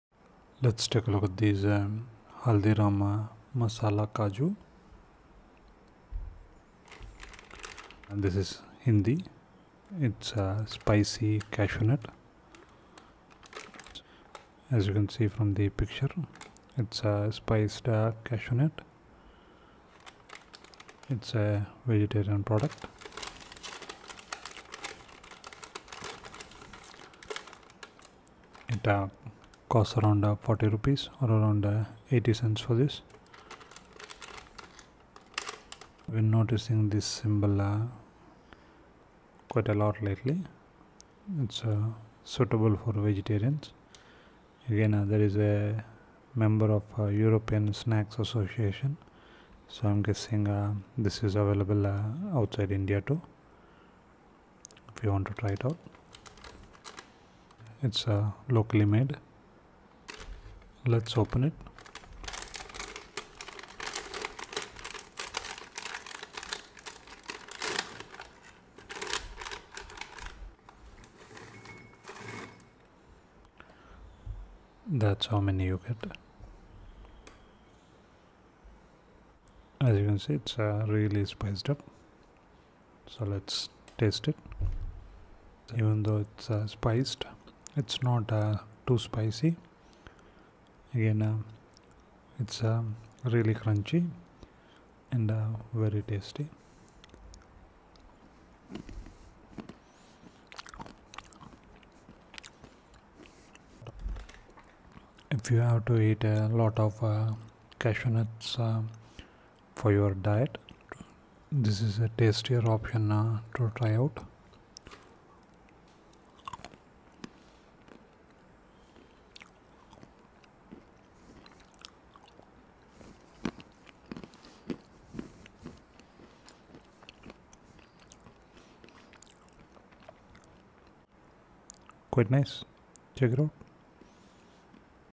Haldiram Masala Kaju
This Masala Kaju is from Haldiram. These spiced cashew nuts are crunchy and spicy. Best way to eat cashew nuts. It costs Rs 40 around 80 cents.